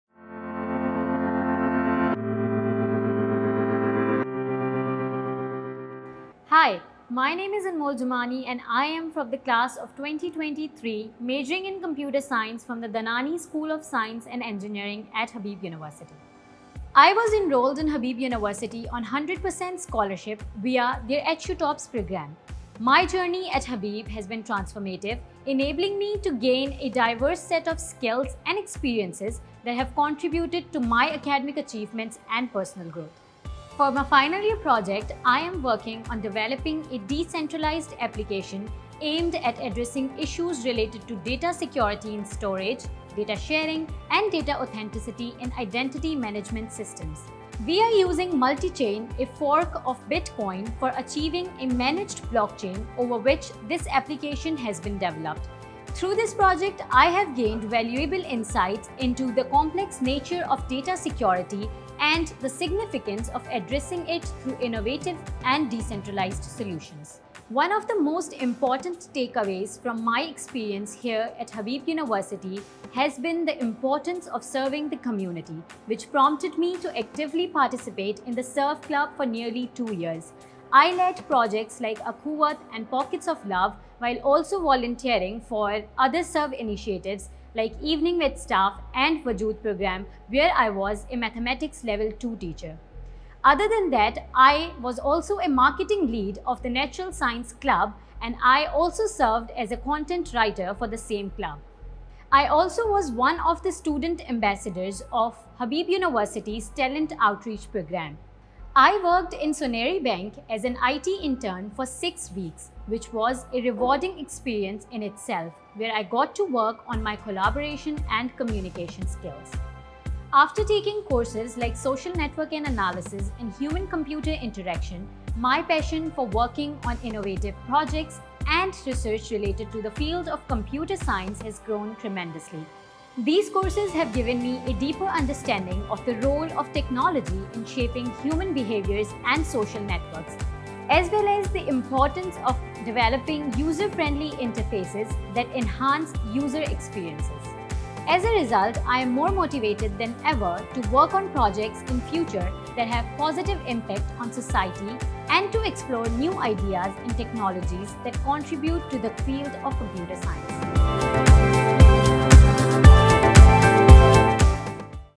Anmol Juman | Computer Science (CS) | HU Stars Class of 2023 | Habib University
Discover how Anmol Juman's academic and extracurricular experiences have influenced her professional proficiency and career objectives.

You can explore more about our grads and their professional achievements